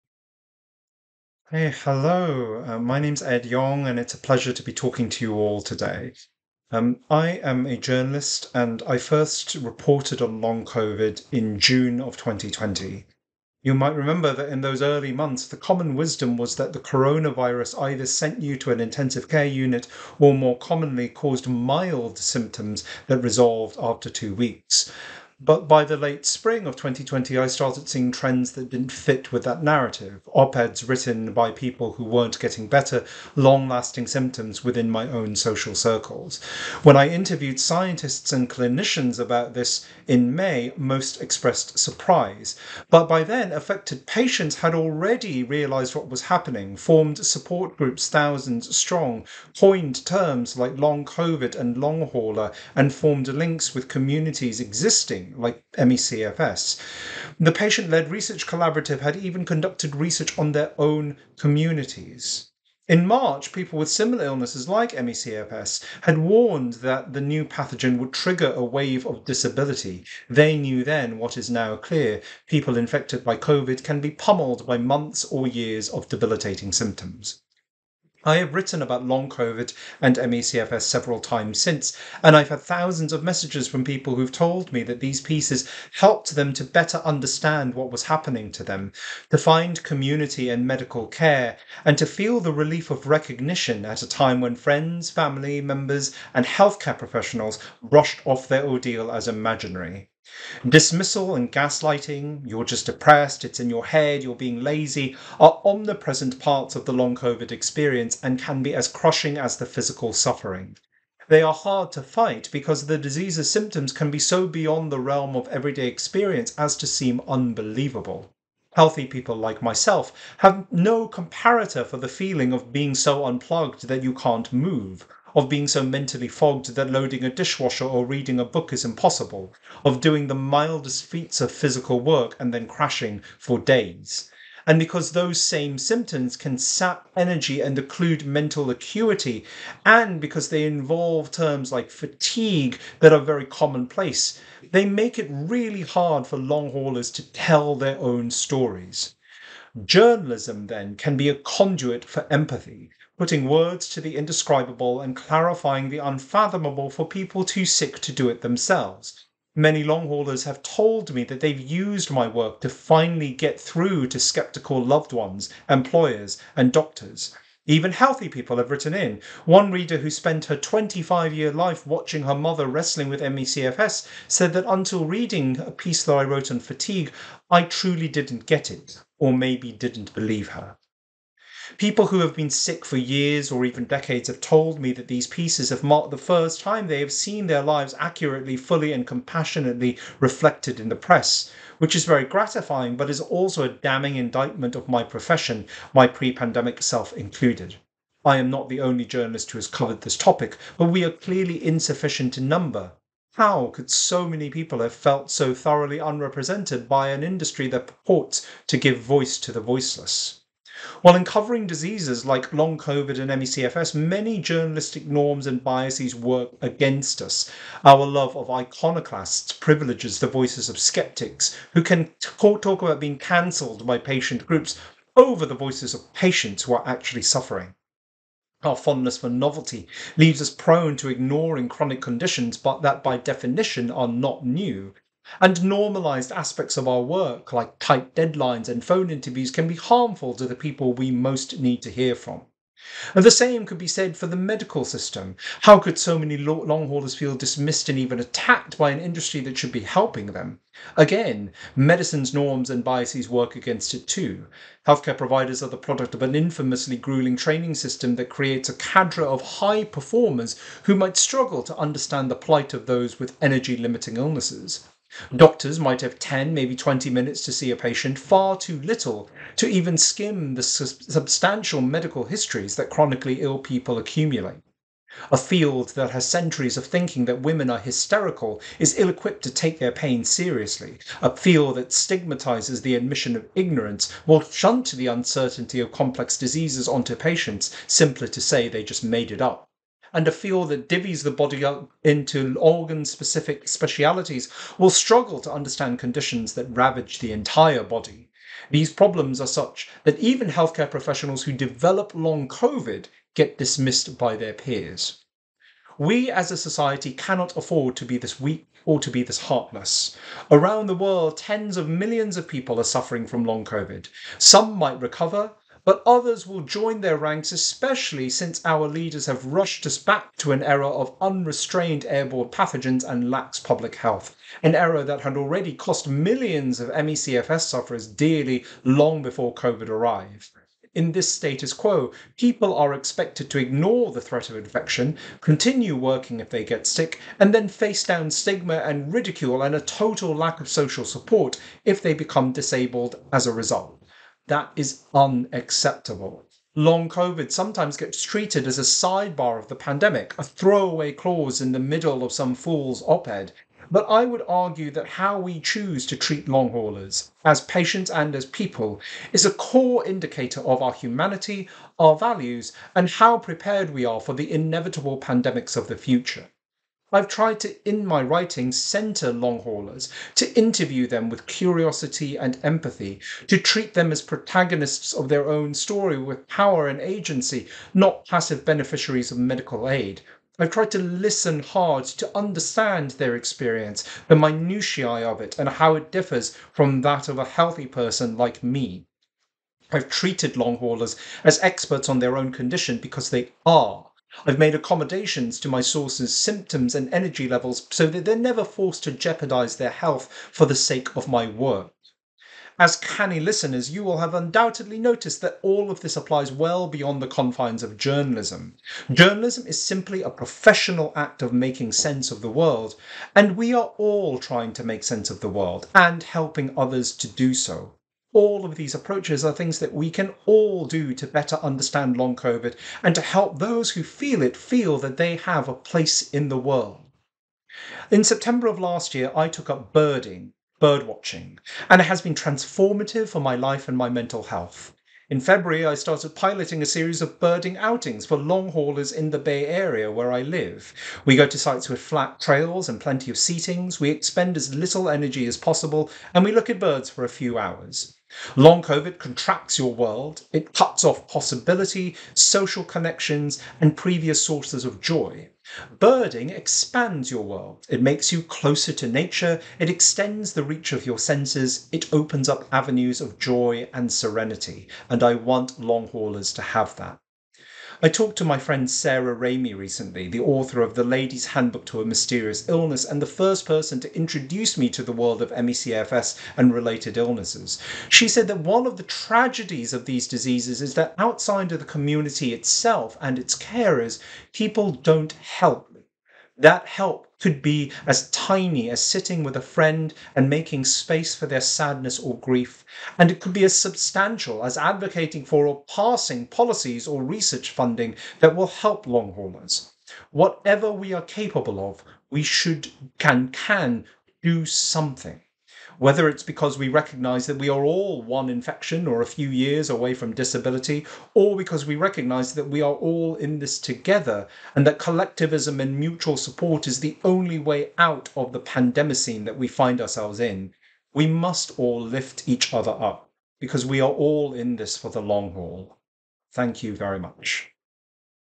Ed Yong: Opening Speech at UniteToFight2024 (Opening Ceremony)
Welcome to the UniteToFight2024 conference! In this opening ceremony, we are honored to present the keynote speech by Ed Yong, Science Journalist and Winner of 2021 Pulitzer Prize.

Thank you for participating in the UniteToFight2024 conference. Your engagement helps drive awareness and support for those affected by Long COVID and ME/CFS.

📝 Subtitles
English transcript: Wiebke K. Schmidt
German translation: Wiebke K. Schmidt
French translation: Gilles Thöni, PhD (@GillesThoni on X/Twitter) and Corinne from Millions Missing France (@MillionsMissgFr on X/Twitter)
Spanish translation: Dr. Leticia Avilés (@DrLAviles on X/Twitter) and @Ekhi_sasienGain (on X/Twitter)